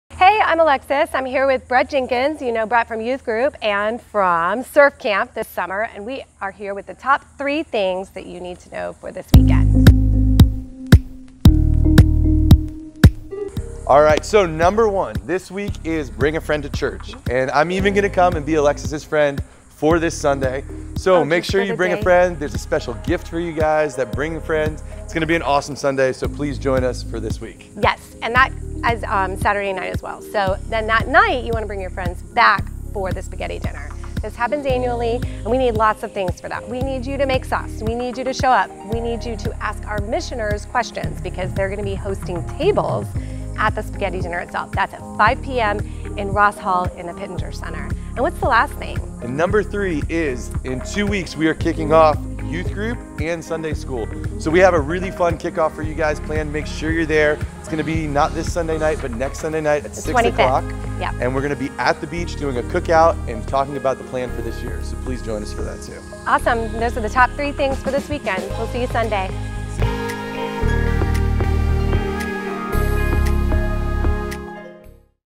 Why would you bring someone to church? We will give you 3 Reasons!
Top 3 Things
1) Bring a Friend to Church
2) Spaghetti Supper
3) Sunday School and Youth Group Aug 25th